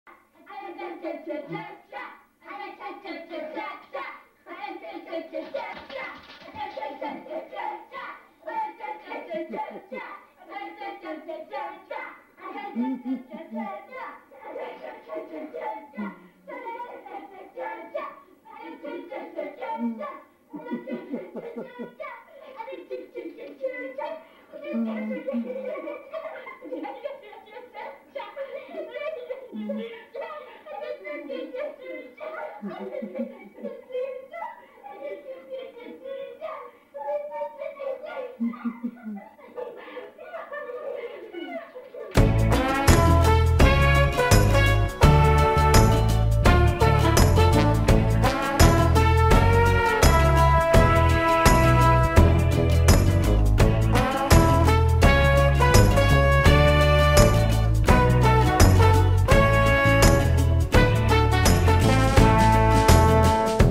Dog dances in a conga line
Dog does the cha-cha with his family.